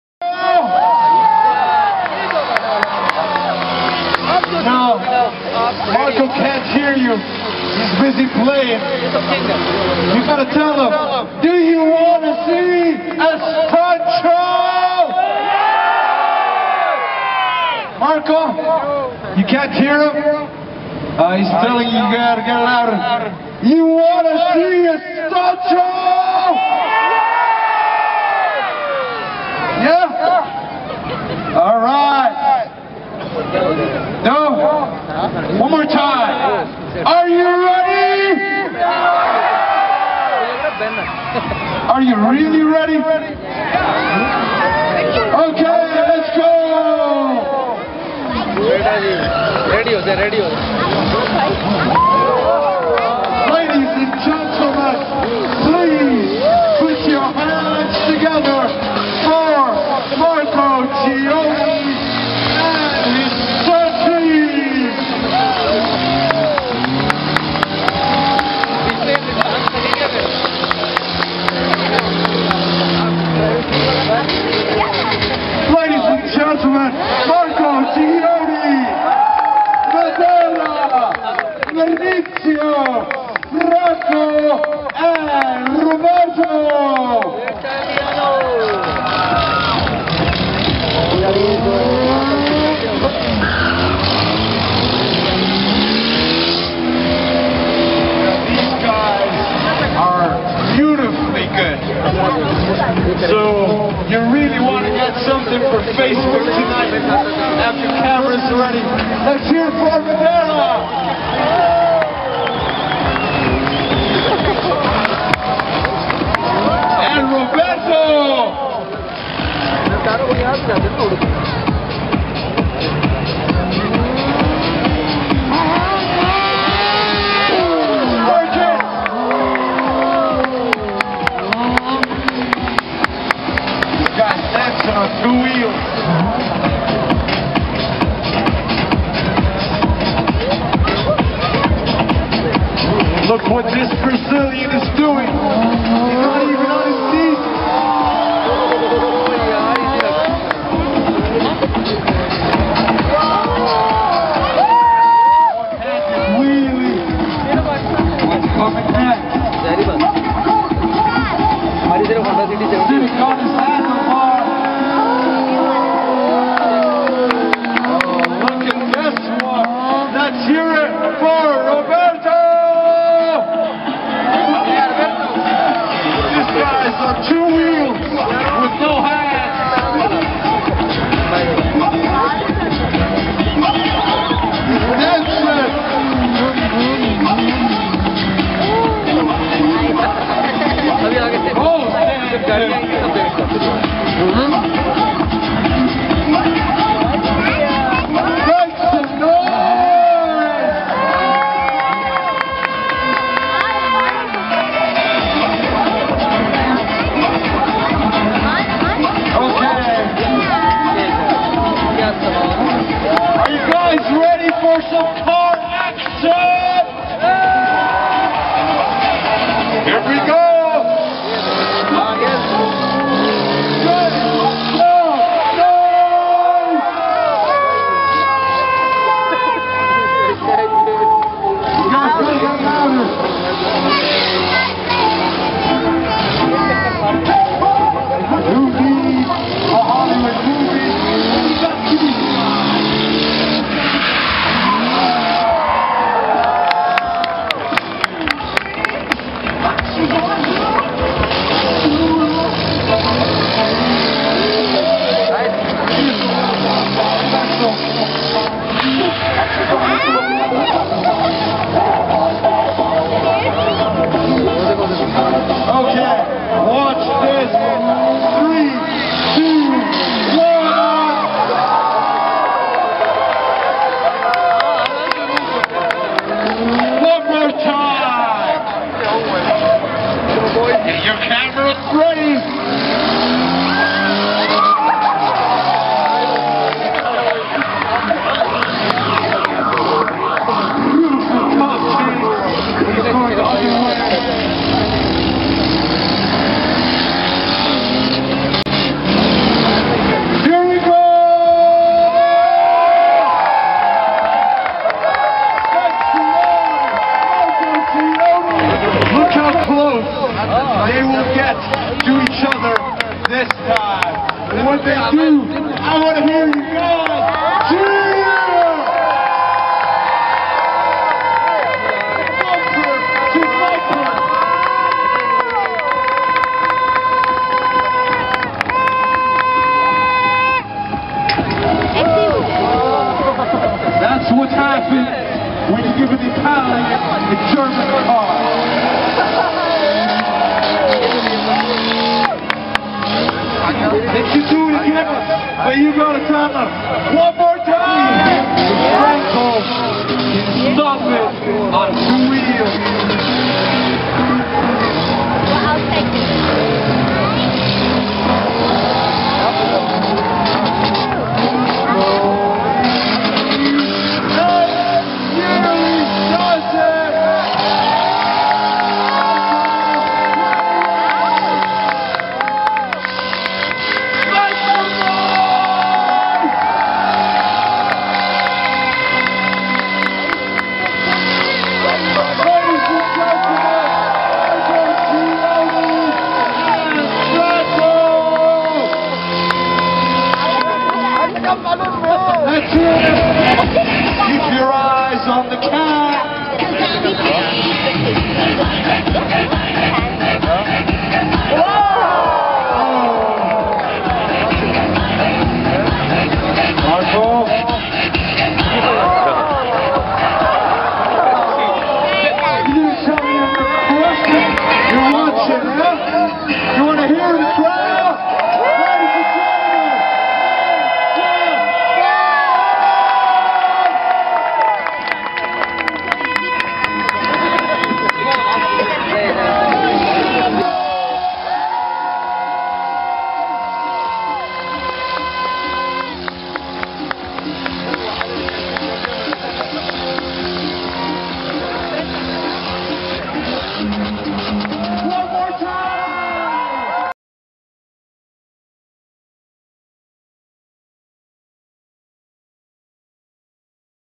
Amazing Car and Bike Stunt Show!
Amazing Car and Bike Stunt Show at the Qatar Motor Show 2013.